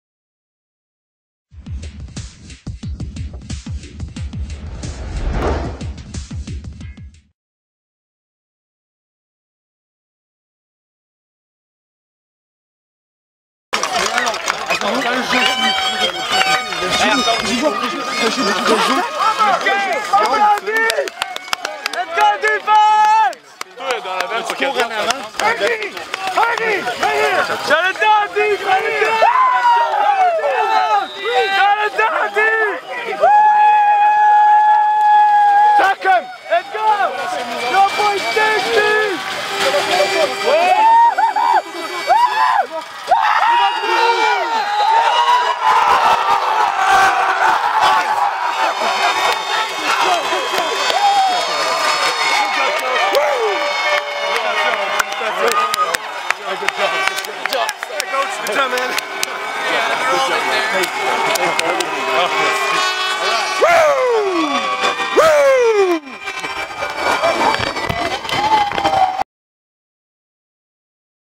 TQC12 | Dernières secondes avant la médaille d'or
Certaines traditions ne se perdent pas. Parmi celles-ci, la douche froide à l'entraîneur-chef qui gagne un championnat est l'une de nos préférées. Brad Collinson n'y a pas échappé, lors de la victoire de Team Québec sur l'Alberta en grande finale de la Coupe Canada 2012.